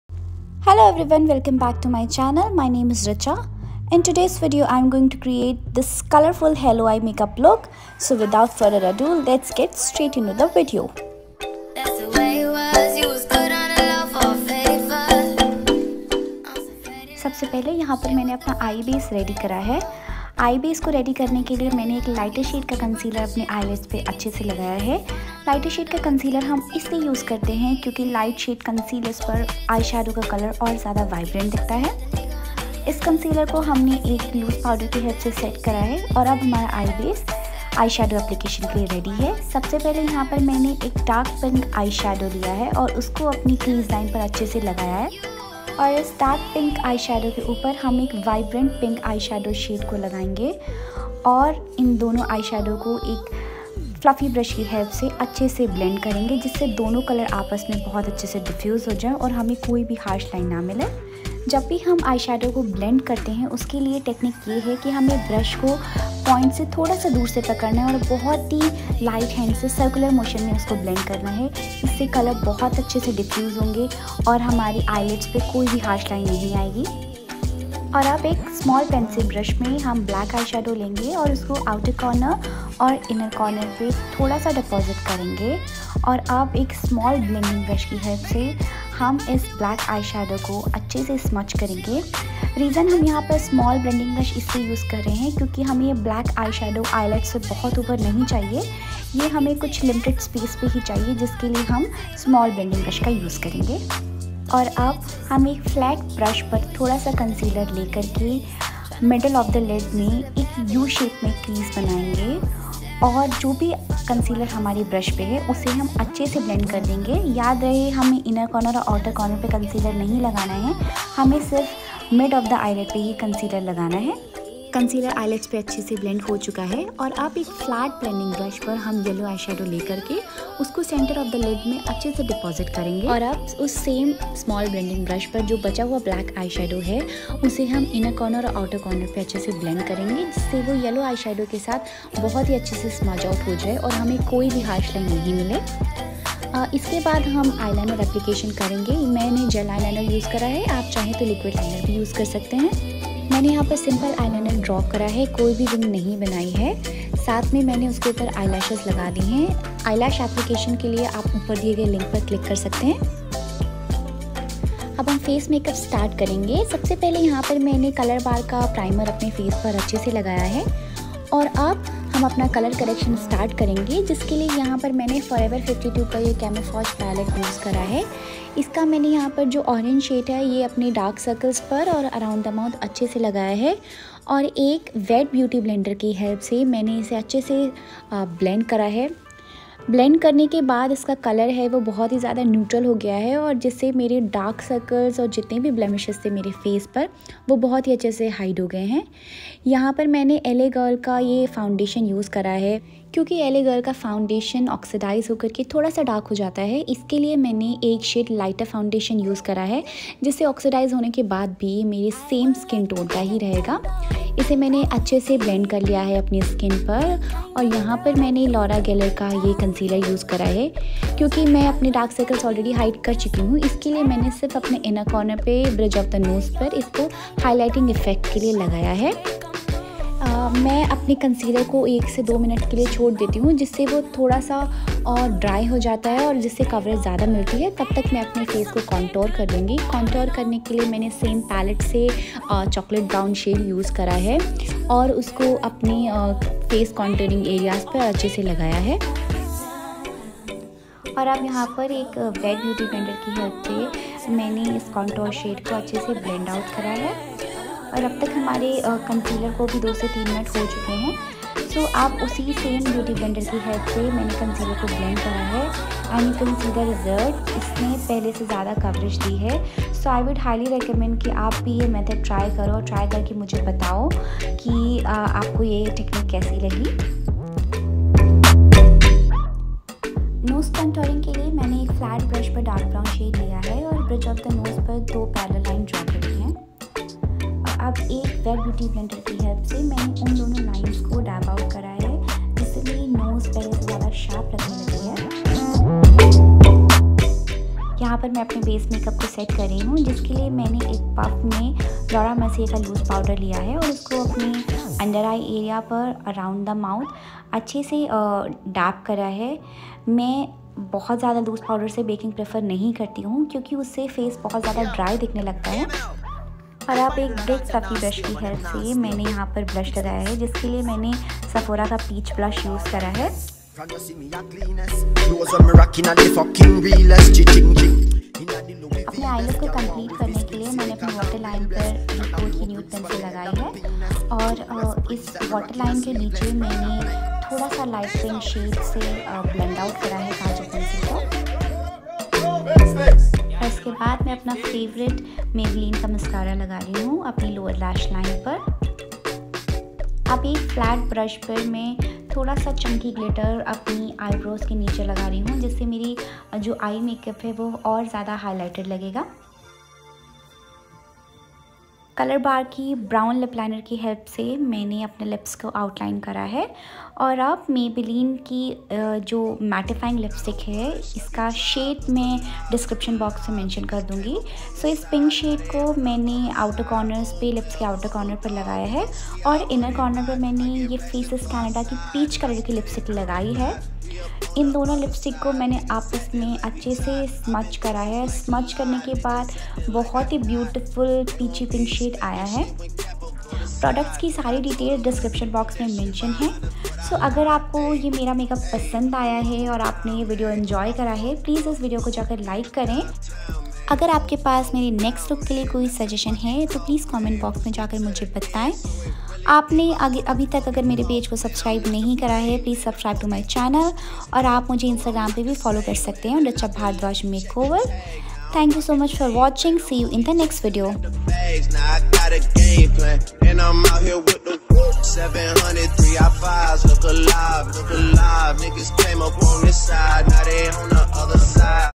Colorful Halo Eyes Makeup Tutorial | PARTY MAKEUP
Hello everyone,

I am a former flight attendant turned into a makeup artist. I create content about Makeup and Aviation.

Love,
Richa

ABOUT THIS VIDEO:

In this video, I take you through the basics of getting ready for a party in a few simple steps. In this video, I will demonstrate to create "Halo Eye Makeup" look.

This is one of my many how-to tutorials in the upcoming series.

PRODUCTS MENTIONED:

Re’quil’ moisturizer
Colorbar primer
Forever 52 camouflage palette
L.A. girl PRO coverage long wear foundation - beige
Laura geller concealer - medium
Laura Mercier loose powder
Anastasia Beverly Hills pomade
Forever 52 gel eyeliner
Maybelline lash sensational mascara
Colorbar lipliner
Faces Canada matte comfy lip shade 10
Maybelline superstay liquid lipstoick - Ruler 80
Too faced highlighter
Anastasia beverly hills Amerzy palette
Nyx small colorful palette
Hudbeauty desert dusk palette
MAC prep and prime setting spray
Brush - London prime and cilios
Lashable lashes - cherry

Copyright-free music.